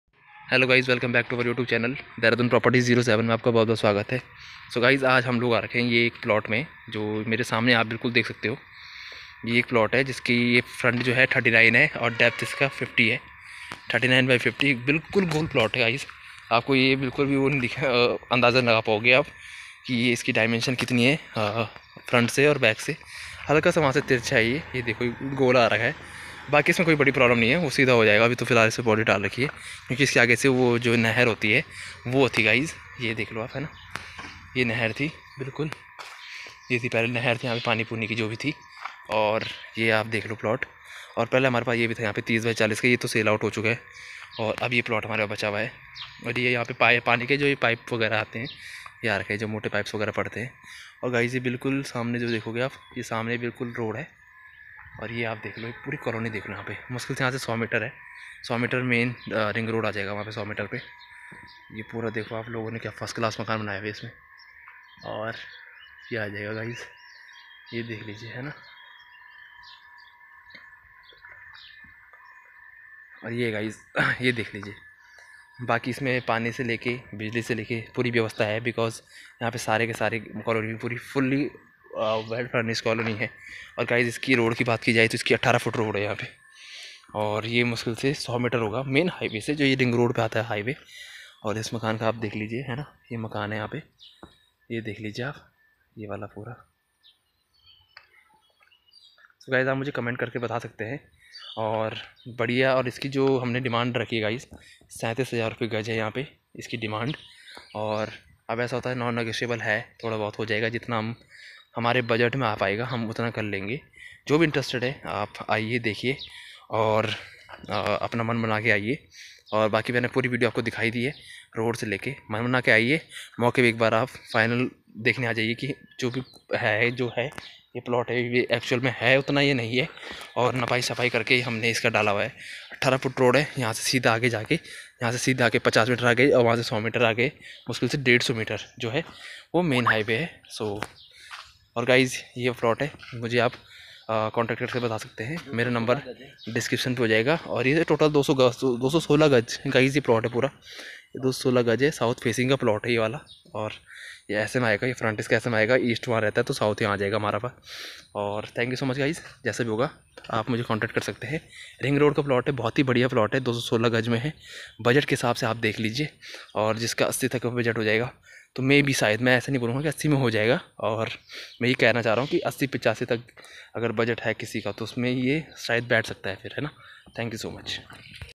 150 meter from main ring road | jogiwala | dehradun
plot size is 39*50 with 150 meter in ring road

plot size is vary comfartable

nearest place jogiwala chok,Sanskar garden ,

school - sgrr,tiny tots, sanskaar international school

hospital - kailash hospital,gupta clinic,

asking price 37000 per gaj